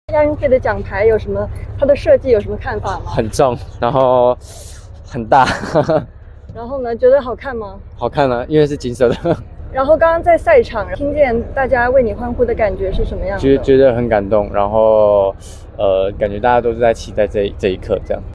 Chinese Taipei judoka wins team's first gold at 19th Asian Games
Chinese Taipei athlete Yang Yung-wei won the gold medal in the men's 60 kg judo competition at the 19th Asian Games in Hangzhou, China on September 24. The gold medal was the first for Chinese Taipei contingent at this year's Asian Games, and is also the 100th gold medal won by the Chinese Taipei team in the history of the Asian Games. Yang's win over South Korea's Lee Harim took place on the eve of his 26th birthday.